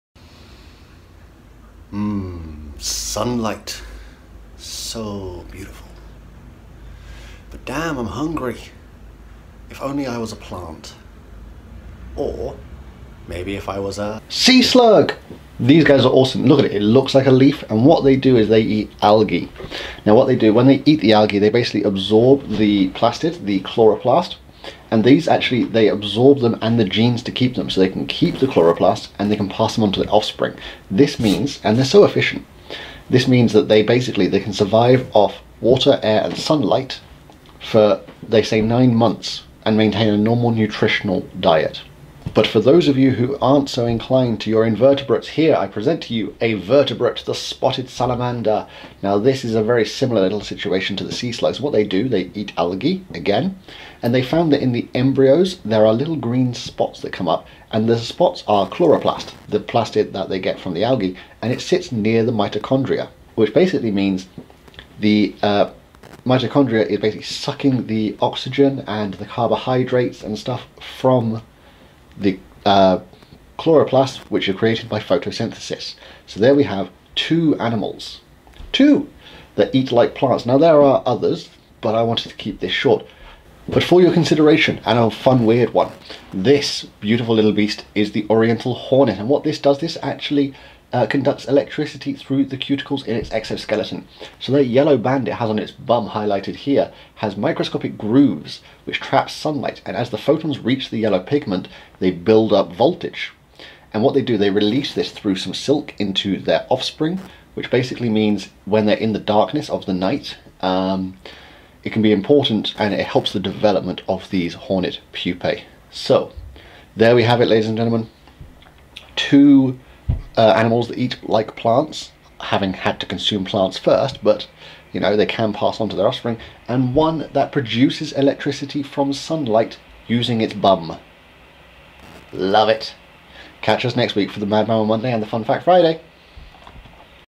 Animal or Plant? A bit of both...? Fun Fact Friday!
Welcome to a Fun Facts Friday! Today is animals that get confused how to eat and copy plants to get their energy. For your consideration we have a sea slug (Elysia timida) the Spotted Salamander (Ambystoma maculatum) and the Orientl Hornet (Vespa orientalis)

Comment and message any animal you wanna see a fun facts about :D